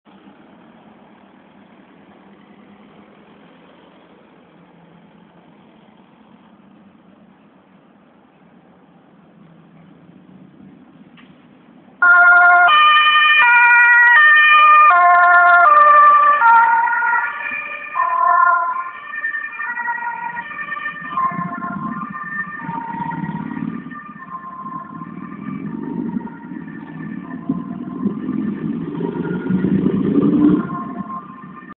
A1 ambulance 25-113 naar een onbekende melding
de 113 met spoed vanaf het Flevoziekenhuis in almere